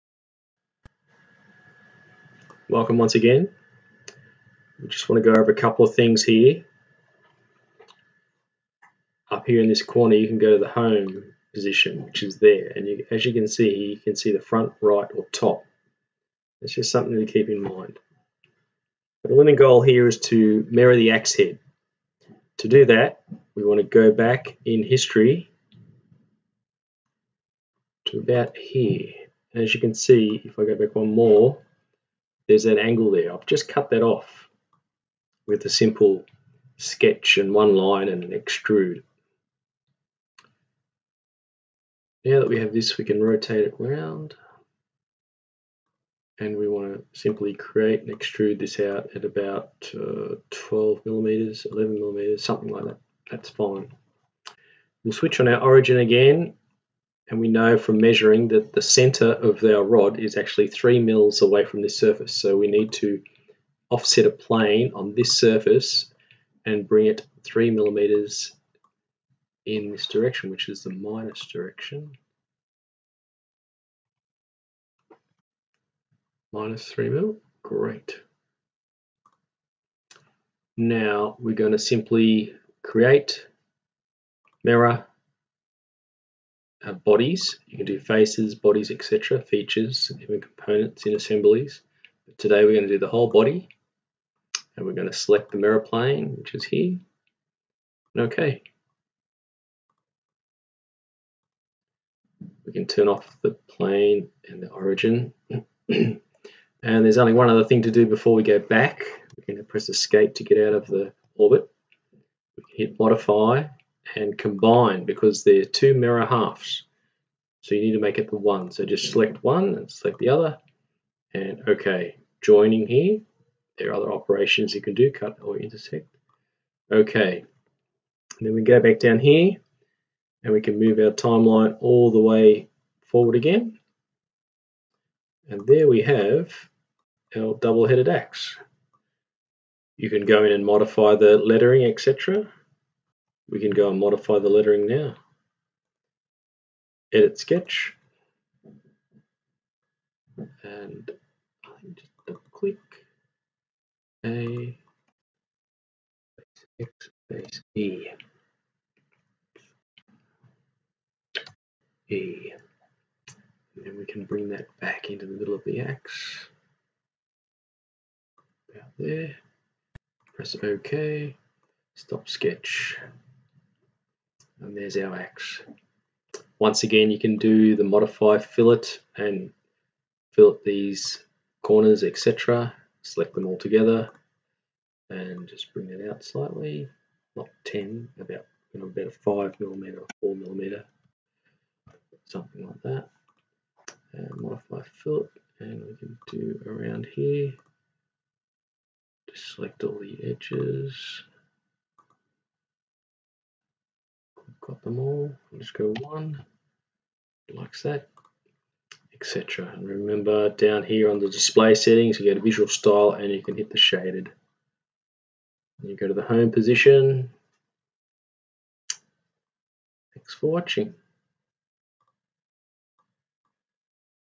Fusion 360 task 7 timeline editing & mirroring
fusion360 learning goals  how to edit in the timeline, mirror and combine
Note: To allow a cleaner mirror operation, you will need to delete the any fillets on the edges before you go back in the time line.